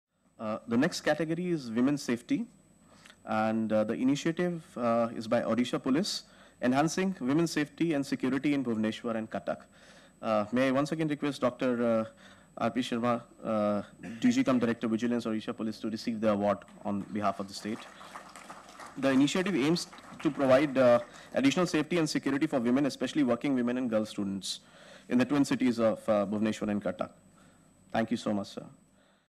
Dr R P Sharma, Odisha Police receiving FICCI SMART Policing Award
Dr. Rajendra Prasad Sharma, DG-cum-Director Vigilance, Odisha Police-Vigilance Directorate receiving FICCI Smart Police Award for Enhancing Women Safety and Security in Bhubaneswar and Cuttack.

The initiative of Odisha Police aims to provide additional safety and security of women, especially working women and girl students in the twin cities of Bhubaneswar and Cuttack. The initiative comprises several innovative measures, leveraging the available resources, communication technology and social media.